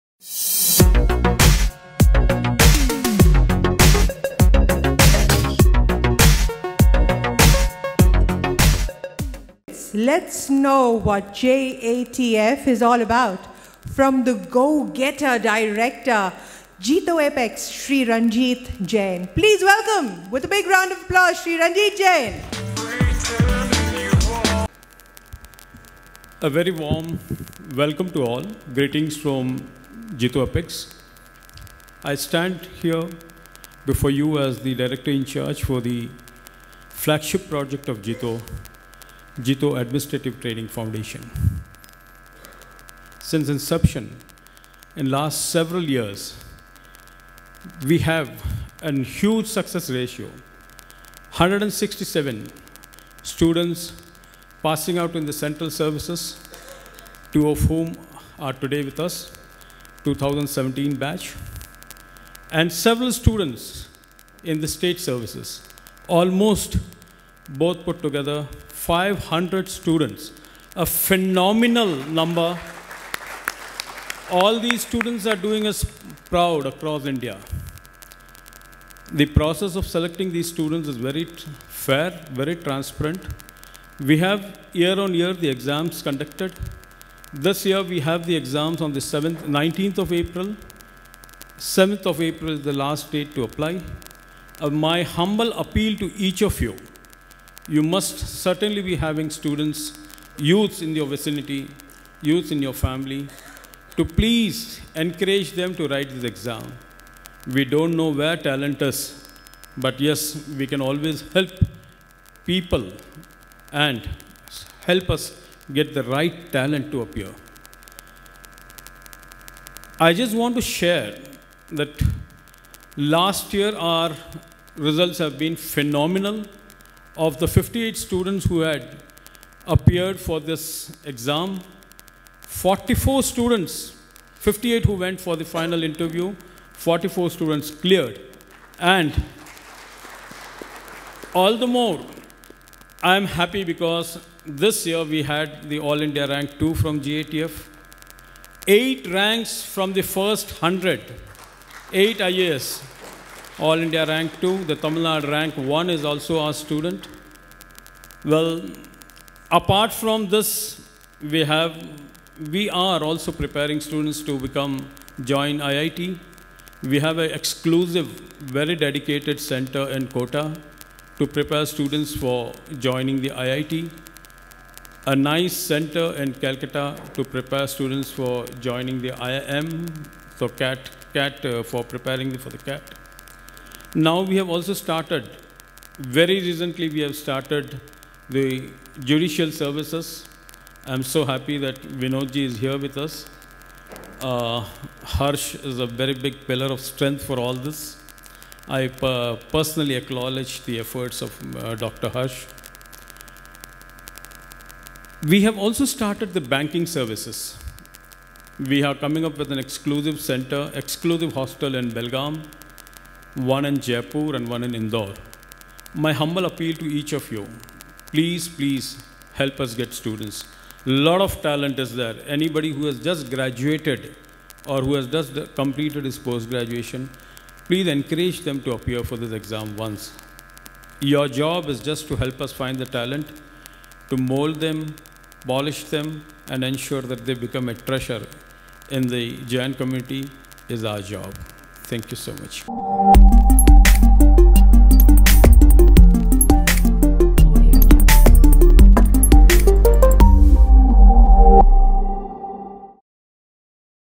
JITO Apex Director Ranjit Jain at JITO Professional Forum Hyderabad - Entrepreneurs Talk
Ranjit Jain Director JITO Apex at JITO Professional Forum Hyderabad